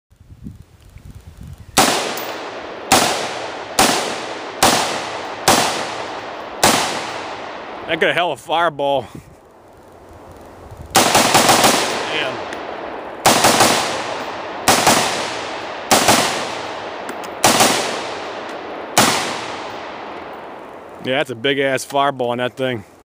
ar-15
brother shooting his ar-15

my brother testing and shooting his ar-15 with new muzzle device.